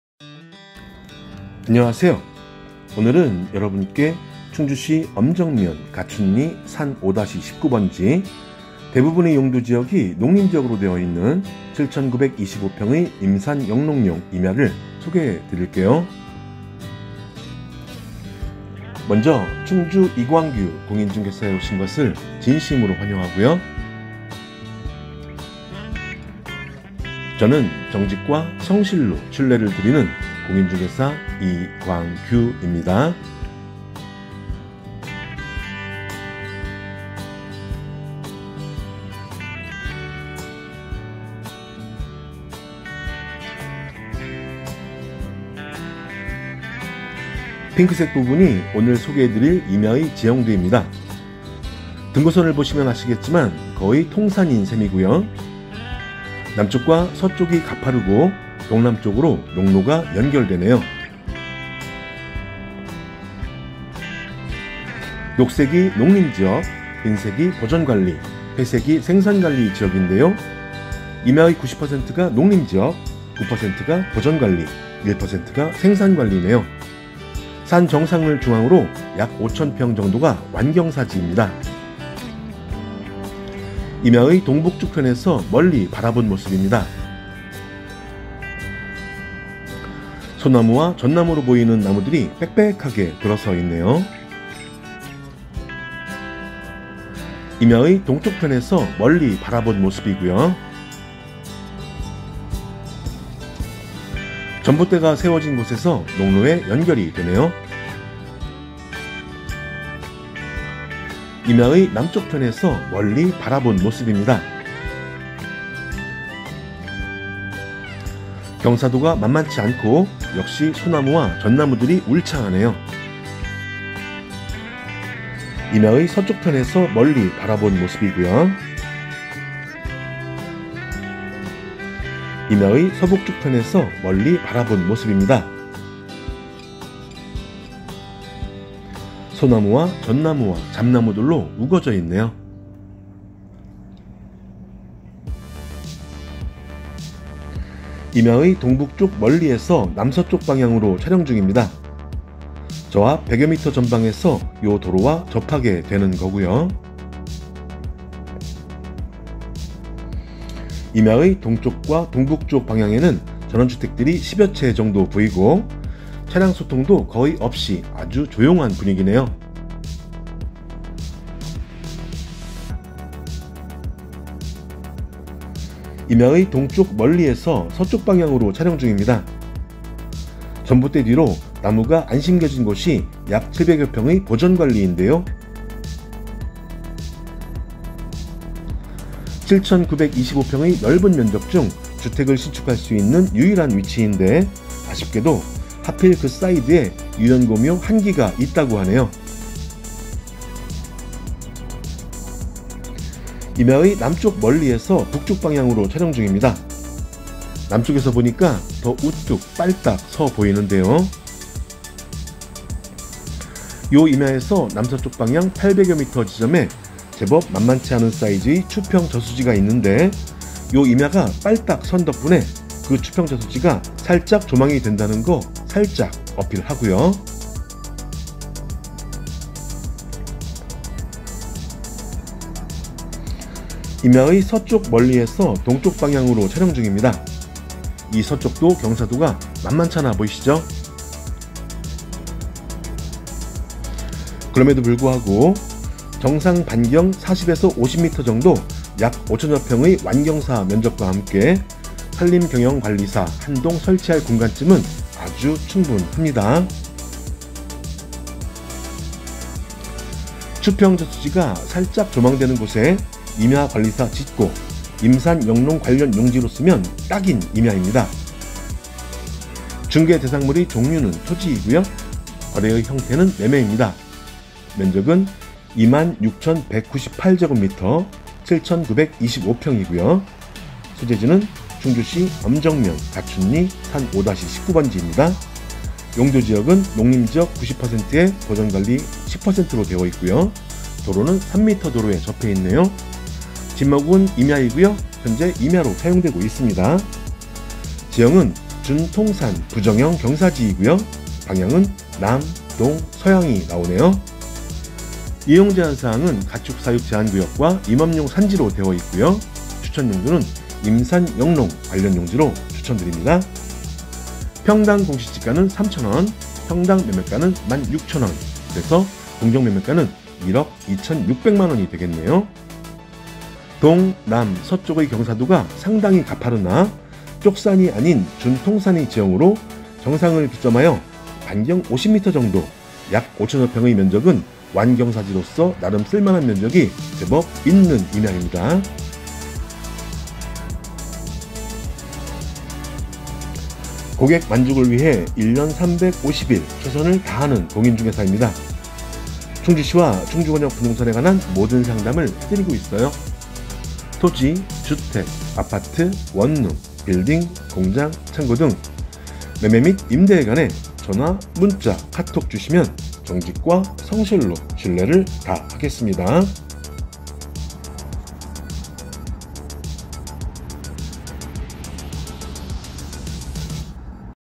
[부동산] 가격상향 임산영농임야 이광규287] 충주시 엄정면 가춘리 산 5-19 임산영농적합임야 7,925평 임야상의 분묘가 처리되면서 1억5천만원으로 가격상향
※ 임산영농용지 ※
충주시 엄정면 가춘리 산 5-19
농림지역(90%) + 보전관리(9%) + 생산관리(1%)
임
3m도로접
7,925평
☞ 1억 5,000만원 ☜

【추평저수지가 살짝 조망되는 곳에 임야관리사 짓고 임산영농관련용지로 쓰면 딱인 임야】
중개대상물 / 토지
거래형태 / 매매
면적 / 26,198㎡ 7,925평
소재지 / 충주시 엄정면 가춘리 산 5-19
용도지역 / 농림90% + 보전10%
도로 / 3m도로접
지목 / 임
사용 / 임야
지형 / 준통산 부정형 경사지
방향 / 남동서향
이용제한사항 / 가축사육제한구역 , 임업용산지
추천용도 / 임산영농 관련용지
평당공시가 / 3,256
평당매매가 / 19,000
공정매매가 / 1억 5,000만원
"동남서쪽의 경사도가 상당히 가파르나 쪽산이 아닌 준 통산의 지형으로
정상을 깃점하여 반경 50m  정도, 약 5,000여 평의 면적은 완경사지로써
나름 쓸만한 면적이 제법 있음 "